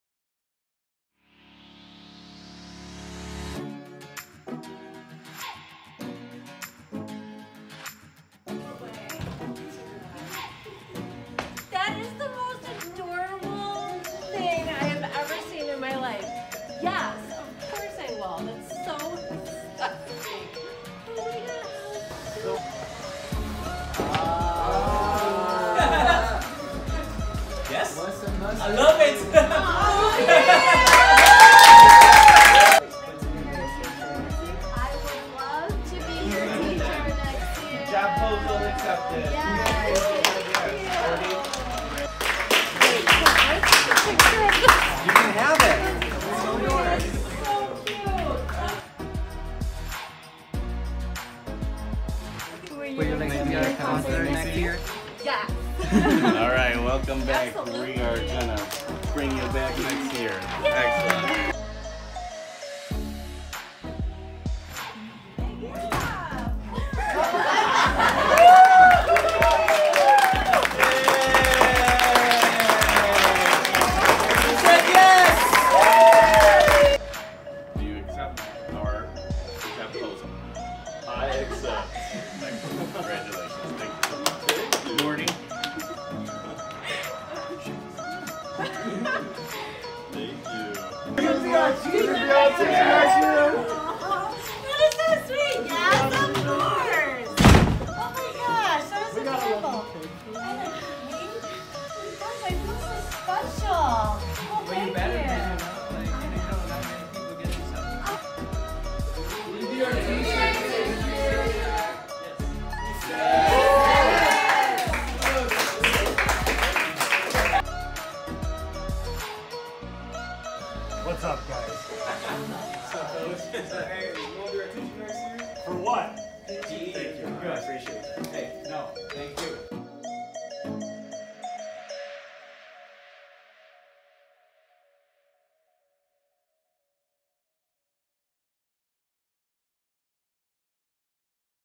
J. Sterling Morton District 201's Teacher Jobposals
On Wednesday, April 11th, the Board of Education approved several first-year teachers to be reemployed this upcoming school year. We wanted to make it a special occasion for the teachers. So, instead of "PROMposals" we invited students to make "JOBposals!"

AND THEY SAID YES!!!